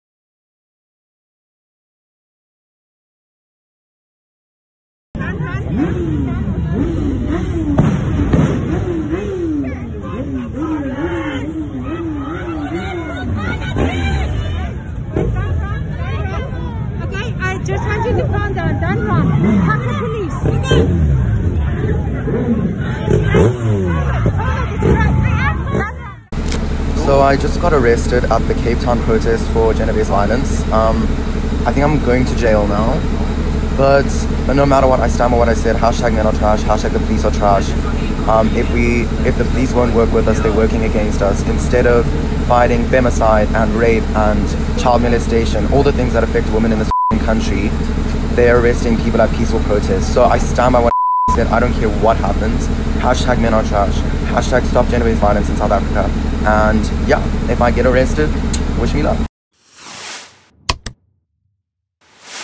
WATCH | Stun grenades unleashed on anti-GBV and anti-crime bikers at Parliament
Police threw stun grendades at a group of mostly women and children, and a mass rally of bikers worried about murders on farms, in a sudden confrontation near Parliament in Cape Town on Saturday.